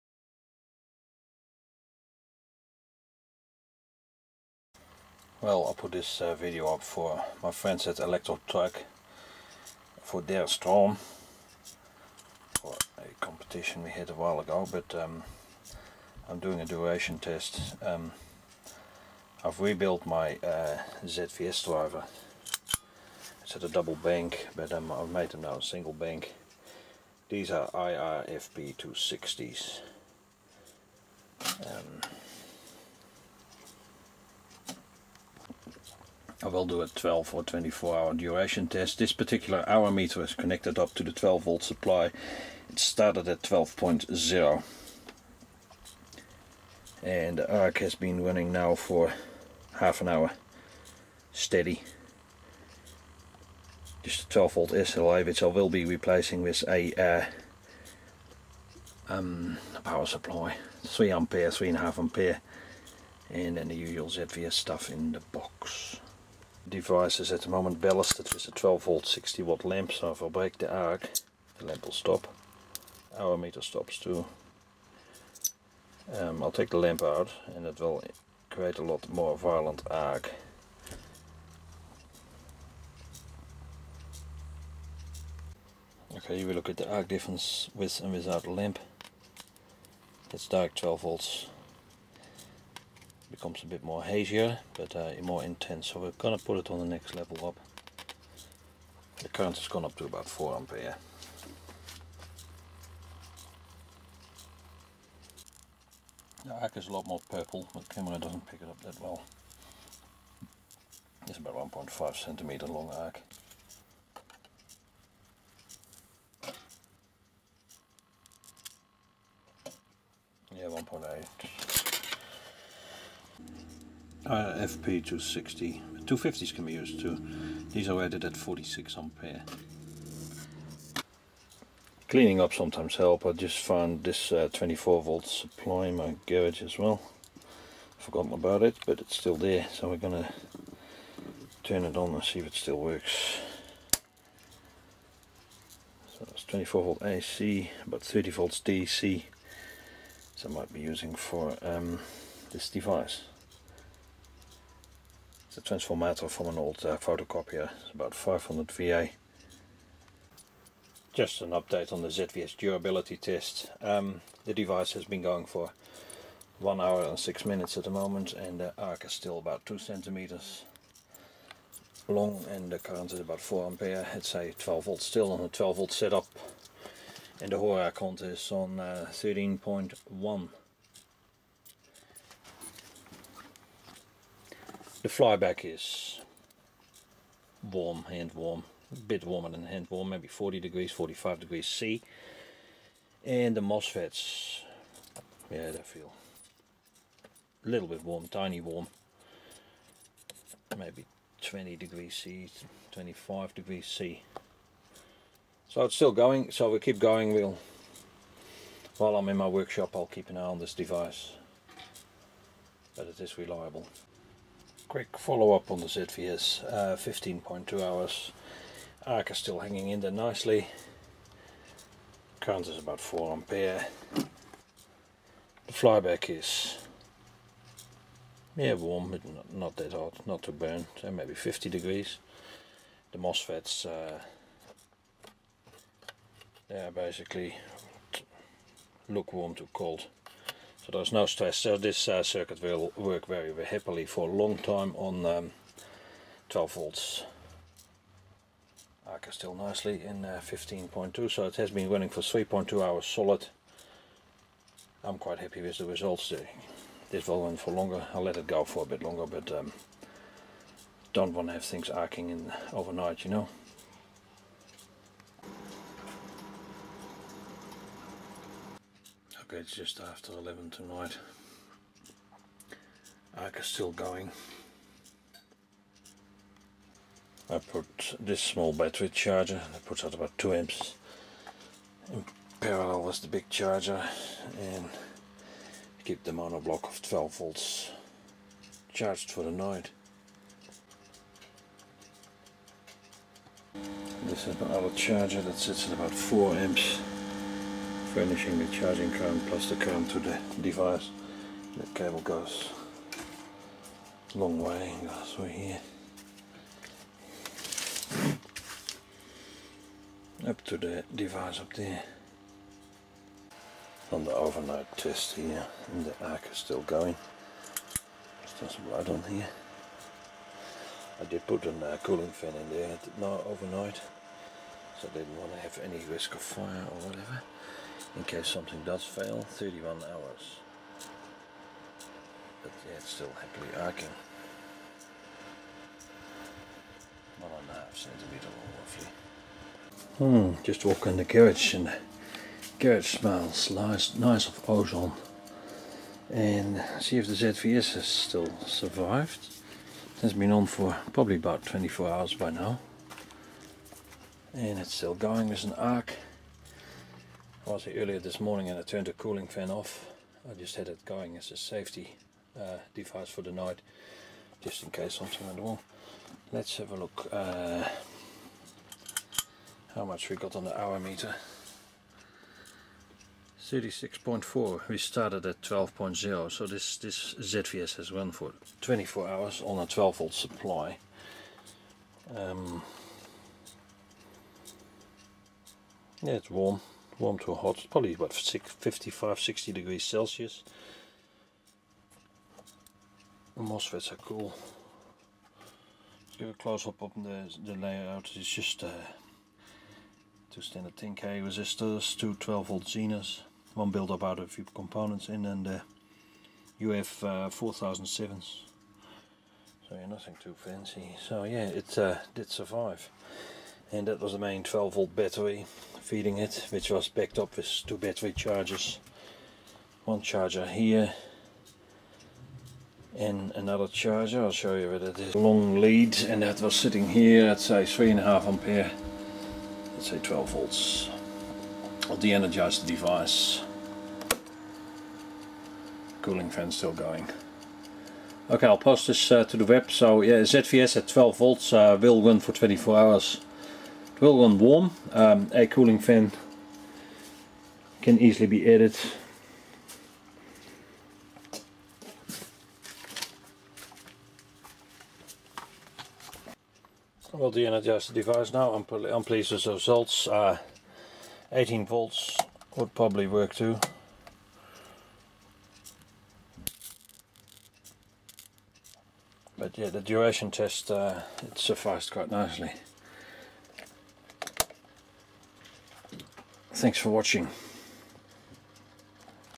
ZVS - 24 Hour durability test
Standard Mozilla ZVS driver test run for 24 hours.

This topic was discussed in Electrotech Forum.

ZVS drivers will work and can draw nice big arcs, but often the flyback transformer fails within a short time due to overheating, or mosfets blow out as I have experienced.

I decided to take up the challenge and run a ZVS driver at 12 Volts 4 Amps for 24 hours.
It survived well. A hour meter was connected to monitor the time the ZVS was running.

The temperature of the IRFP260 mosfets was about 25 to 30°C (77 to 86°F)
The flyback transformer heated up to about 60°C (140°F) without cooling. with forced cooling it dropped to about 40°C (104°F)